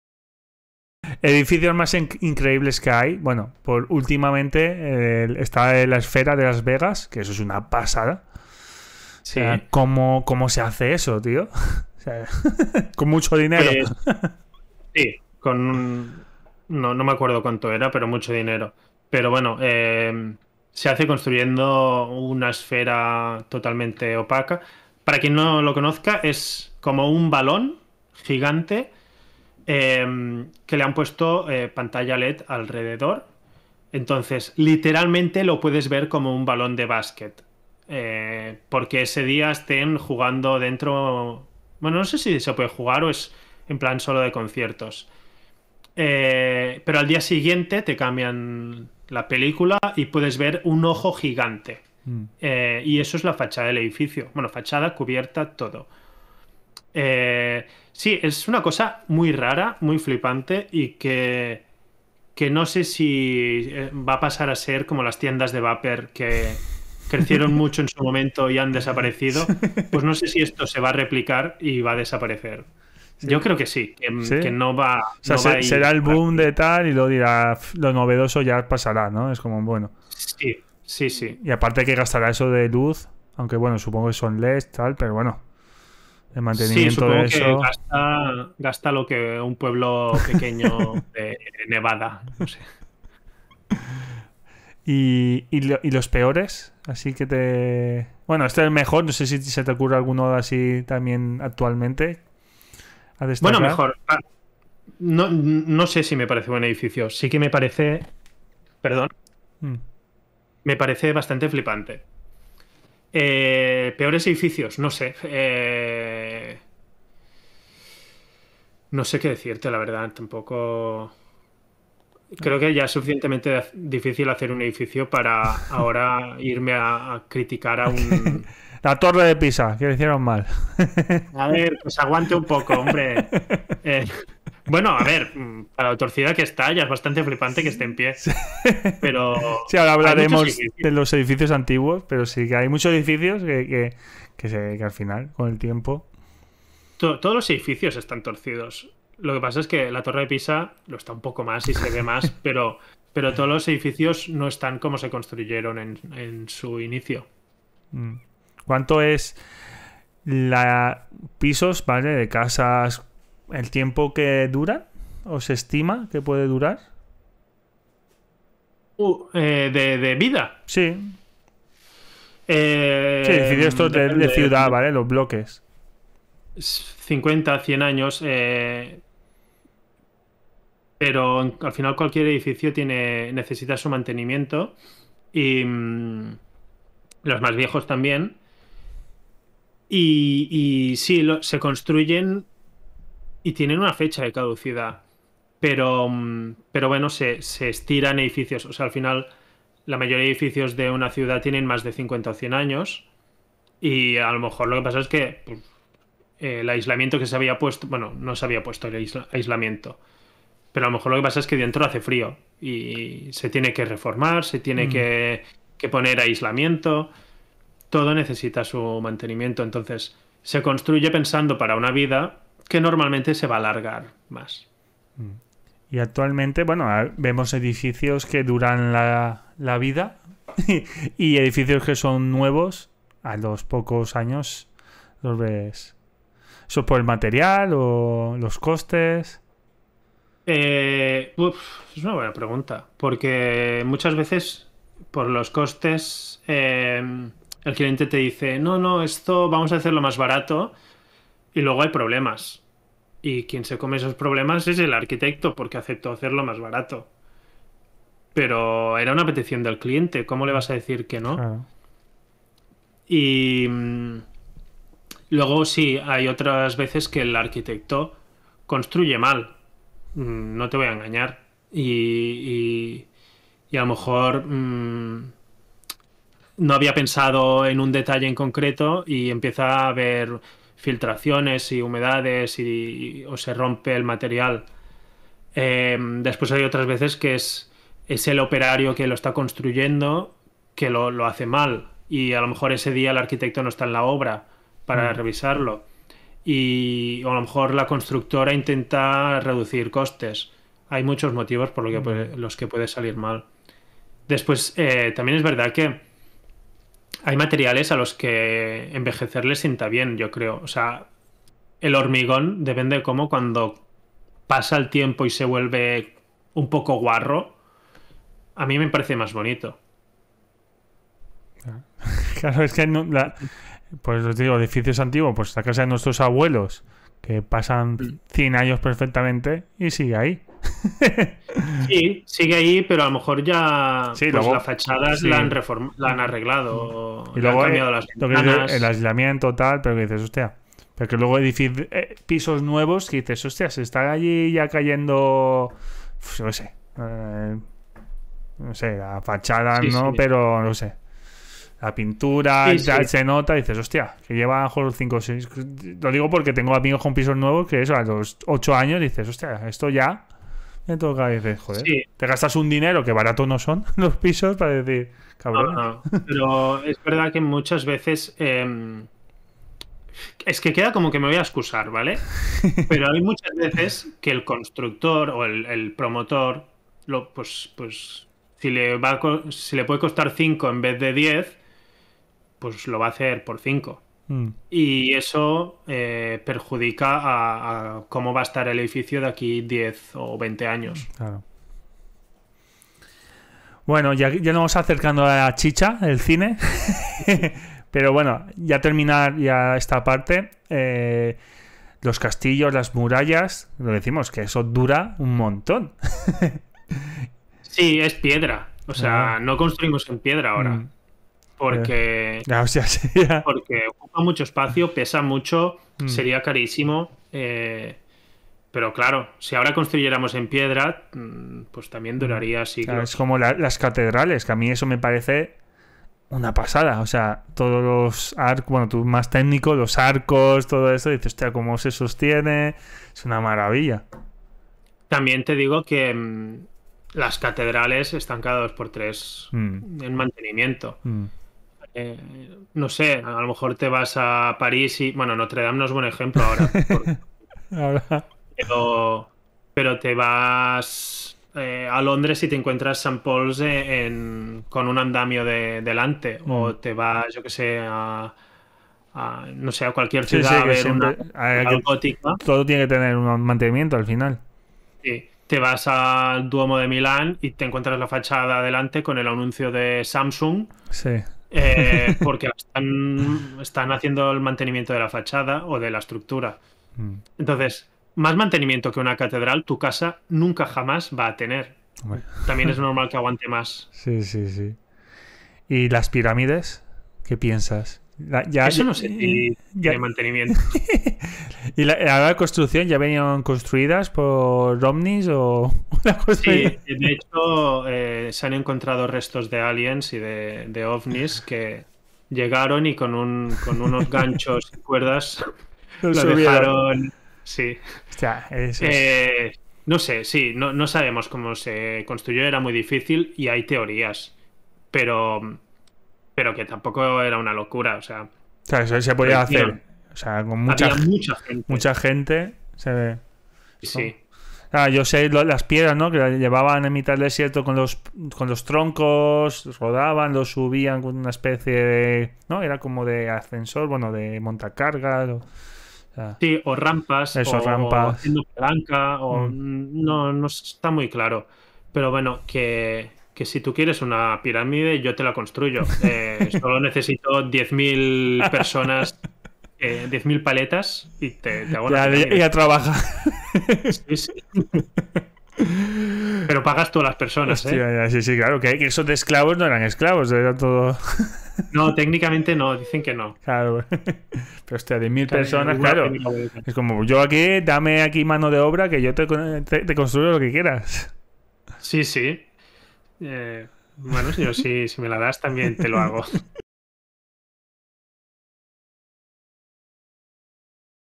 MEJORES edificios @PauMJust #60
Bienvenid@ al programa 60 de El Fotomatón. Pau M. Just, es un arquitecto que comenta la arquitectura en el cine.